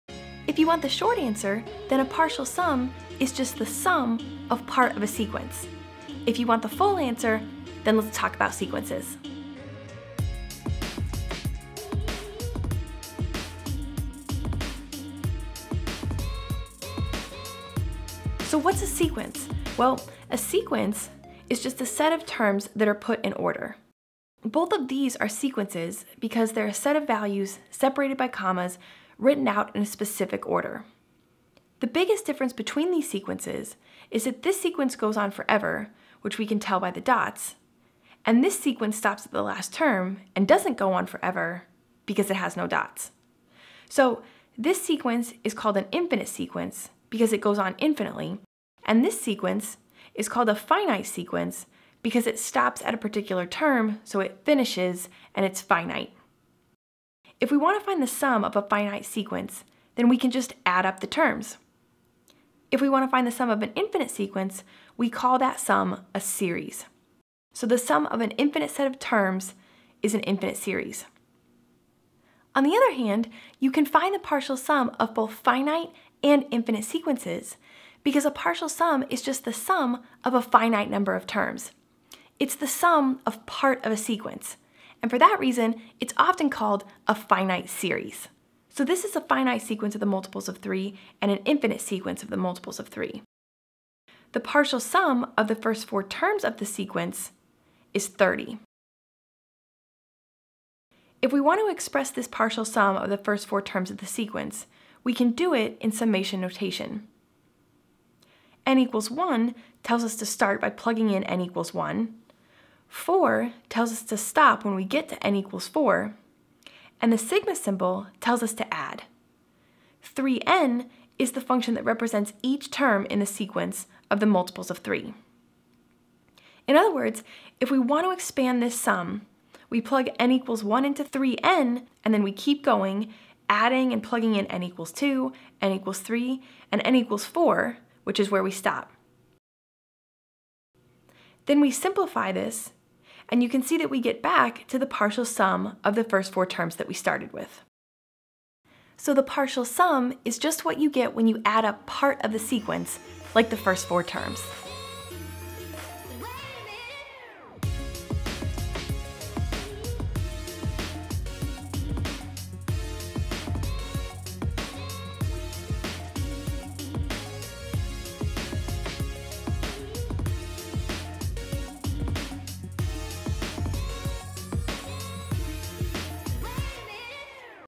What are partial sums?
Ever wondered what a partial sum is? The simple answer is that a partial sum is actually just the sum of part of a sequence. You can find a partial sum for both finite sequences and infinite sequences.

When we talk about the sum of a finite sequence in general, we’re talking about the sum of the entire sequence. Because the sequence is finite, we can find the sum of the entire thing by adding all the terms together. When we talk about the sum of an infinite sequence, we’re talking about the sum of the entire sequence. But because an infinite sequence goes on forever, we have to use special tricks to find the sum of the whole thing. So we use a special name for the sum of an infinite sequence; we call it a “series”.

A partial sum, on the other hand, is just the sum of part of a sequence. In other words, we just take the sum of a few terms of the sequence to find the partial sum. For that reason, we can find the partial sum of either a finite sequence or an infinite sequence. Oftentimes, you’ll be asked to find the partial sum of something like the first five terms of the sequence. In which case, you simply add the first five terms together, and you’ve got the partial sum of the first five terms of the sequence.

0:18 // What is a sequence? What’s the difference between a finite sequence and an infinite sequence? How do you find the sum of a finite sequence or an infinite sequence?
1:12 // What are partial sums? What kind of sequence do you need in order to find a partial sum?
1:41 // How do you express a partial sum in summation notation? How do you expand the summation notation to show the partial sum?